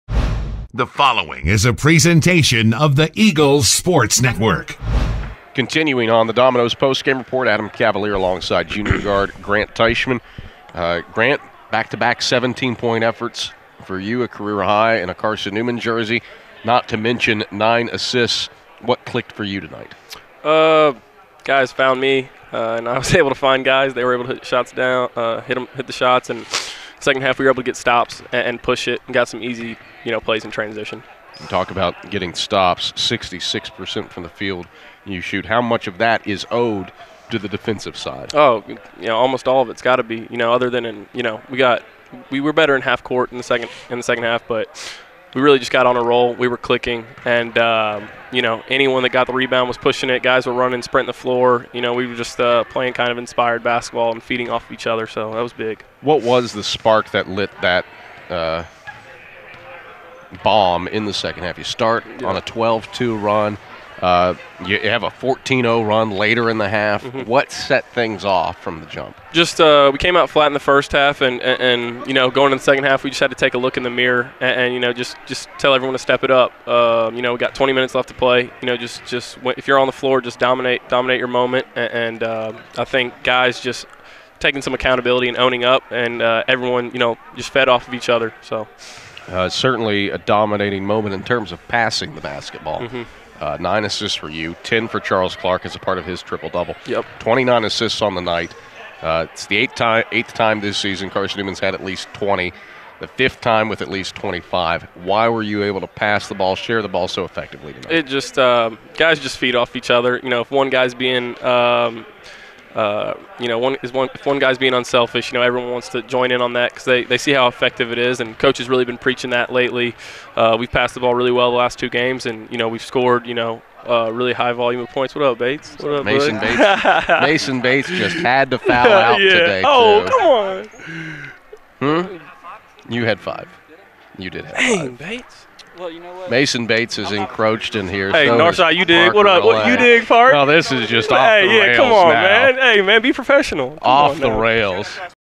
Carson-Newman Basketball: Grant Teichmann recaps Anderson 1-31-18
Grant Teichmann matched a career high with 17 points and handed out nine assists.